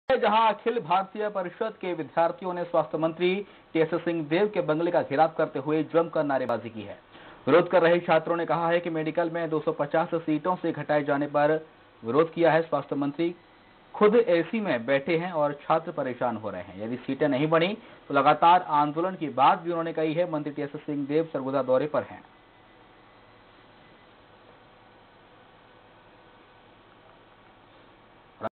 रायपुर: ABVP विद्यार्थियों ने स्वास्थ्य मंत्री टी एस सिंहदेव बगंले का घेराव कर जमकर नारेबाजी की..
रायपुर में अखिल भारतीय परिषद के विद्यार्थियों ने स्वास्थ्य मंत्री टी एस सिंहदेव बगंले का घेराव कर जमकर नारेबाजी की..